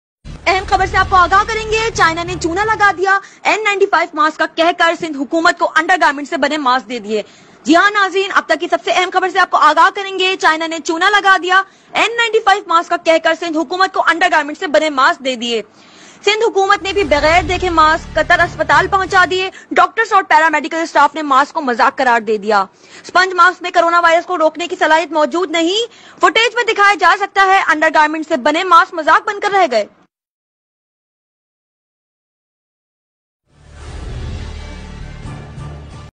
Pakistani anchor "China ne choona laga diya" China send underwear as N95 mask | Corona Virus be game
China send underwear as N95 mask to Pakistan said by the Pakistan anchor